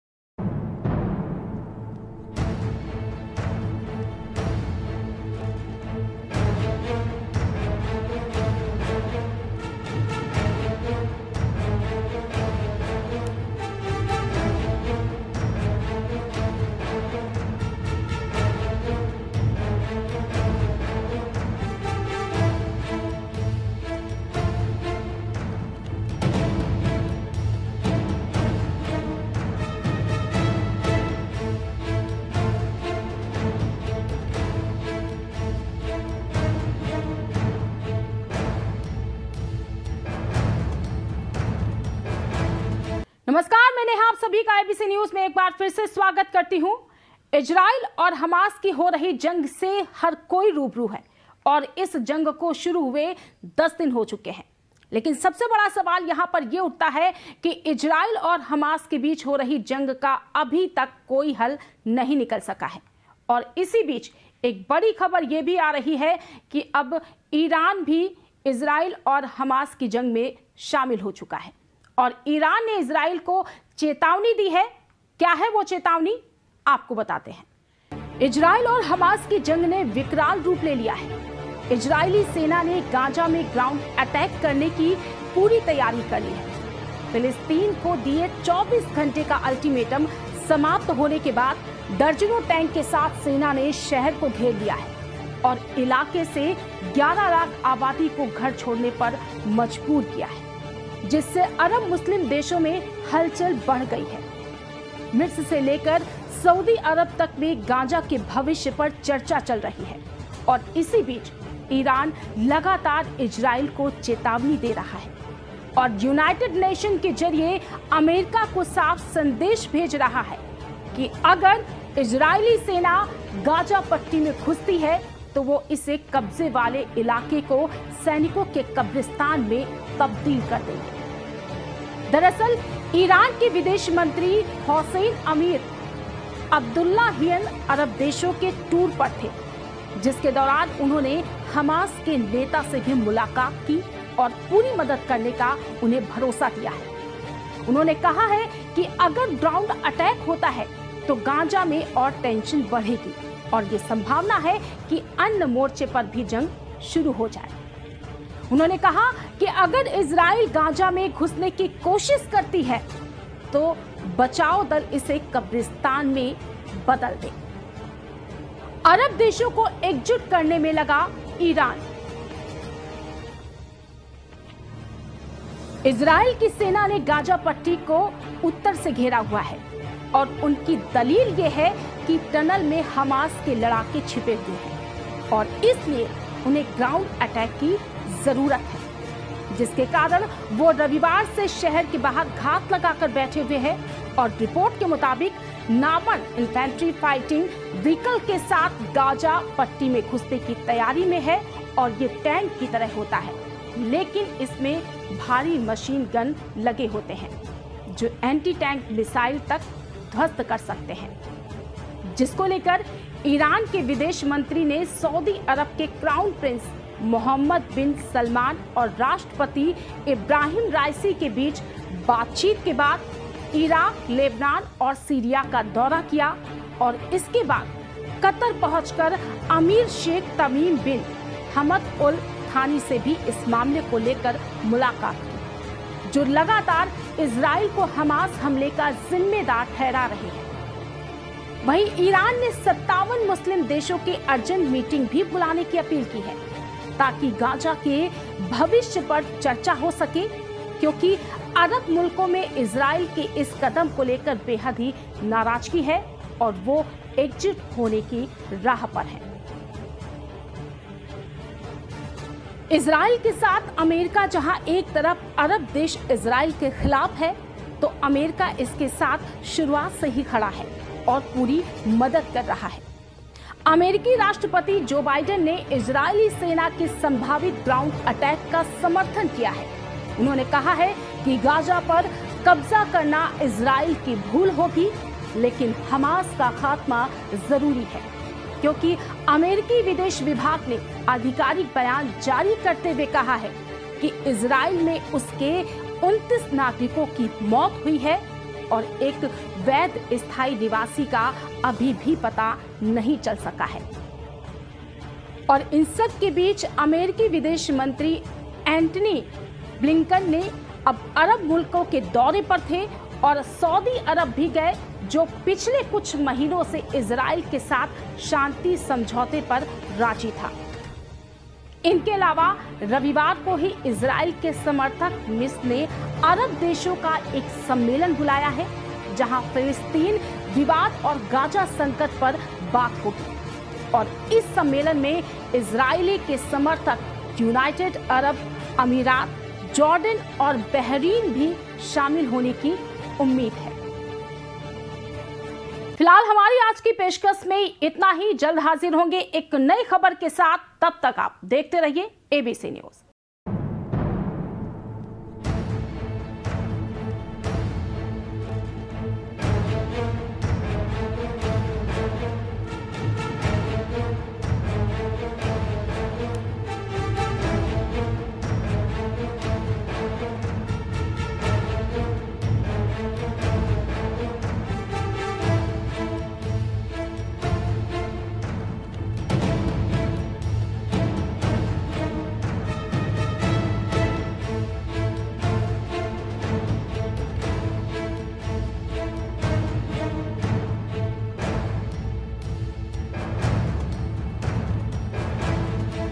Iran की Israel को दी जंग की धमकी | War | SPECIAL REPORT
इजरायल के हमास के खिलाफ चल रही जंग के बीच ईरान की नाराजगी  दुनिया को एक नए तनाव को जन्म दे सकती है।  ईरान लगातार इजरायल को चेतावनी दे रहा है कि अगर हमास पर हमले नहीं बंद हुए, तो उसे  इस JUNG  में उतरना पड़ सकता है। ईरानी विदेश मंत्री अभी हाल ही में लेबनॉन गए थे. और हिजबुल्लाह के हमले की धमकी भी देकर आए। दरअसल हिजबुल्लाह एक शिया आतंकी संगठन है। जिसका इजरायल के साथ युद्ध हो चुका है। और अब ईरान न सिर्फ हिजबुल्लाह और हमास को शह दे रहा है, बल्कि खुद भी खुलकर सामने आने की धमकी दे रहा है। ऐसे में यह चर्चा शुरू हो गई है। कि अगर ईरान भी इस युद्ध में उतरता है, तो क्या होगा ?

DEN- 157
SITI –85
JGD-99